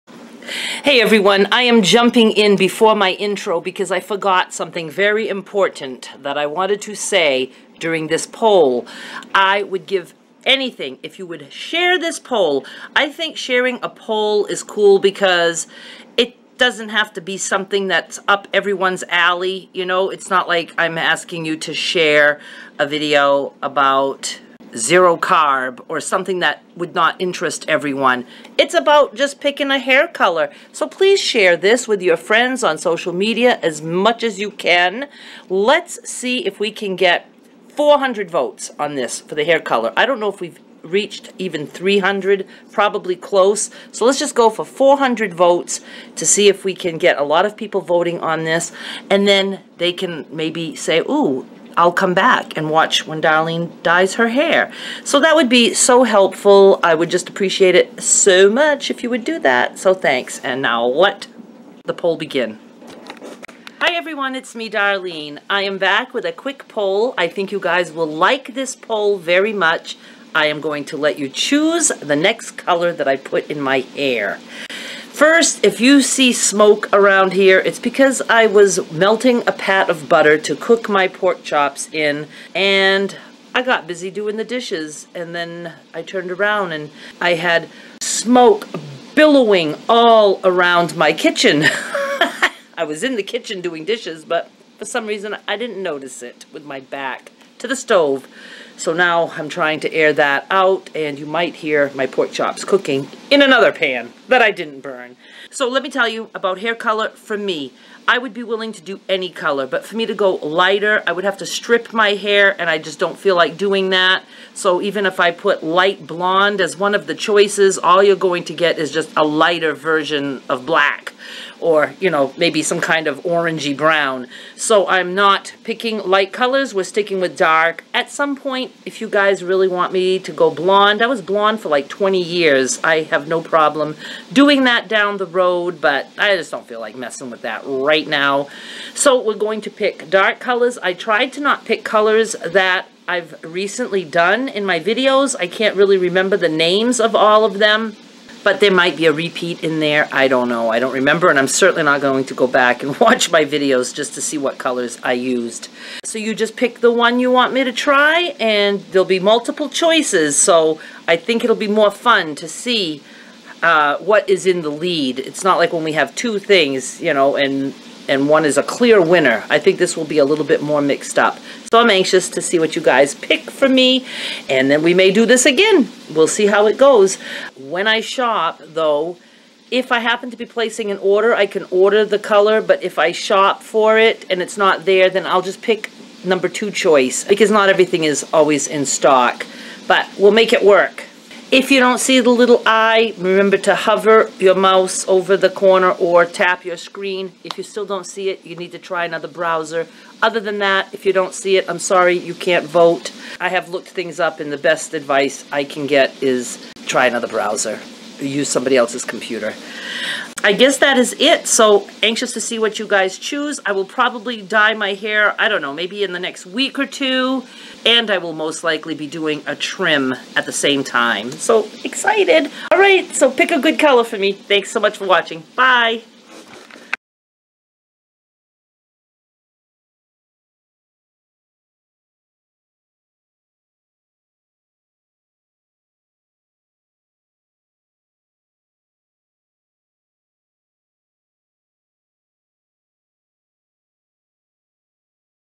HAIR COLOR POLL [CLOSED] - What Color Should I Dye My Hair Next? Please Vote!
Just a quick poll for you to pick my next hair color!  Thanks so much for participating!  Please share this poll with your friends and remind them to watch me dye my hair after I buy the color you choose.  Do love an audience with me in my bathroom! lol